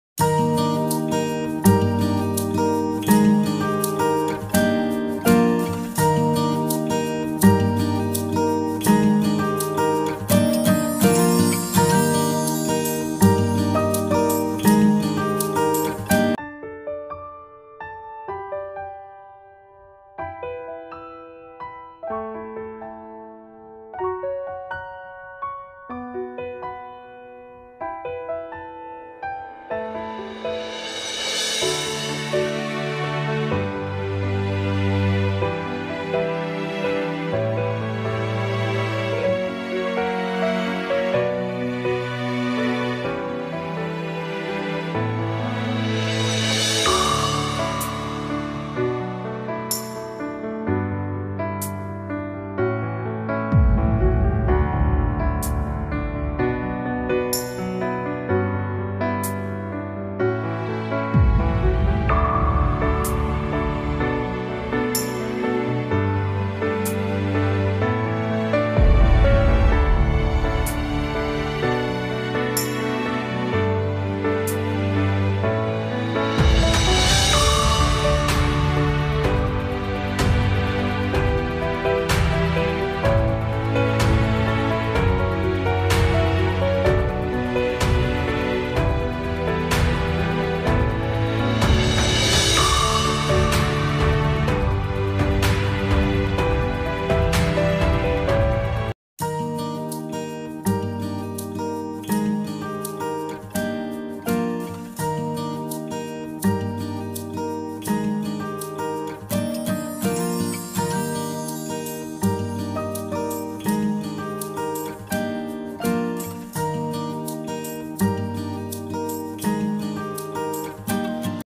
Write an application to the headmaster for setting up a canteen on your school campus
Hope you like this video, so don't forget to do a like👍, share, comment💬 in this video.